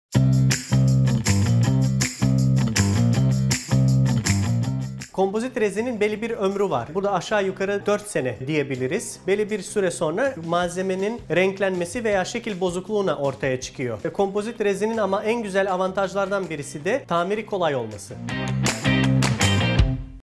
Kompozit Lamina Uygulaması - Dr. Jusuf Lukarcanin
Medicana International İzmir Hastanesi Ağız ve Diş Sağlığı bölümünden Dr. Jusuf Lukarcanin ''Kompozit Lamina Uygulamalarında Karşılaşılan Sorunlar Nelerdir?'' sorusunu sizler için yanıtladı.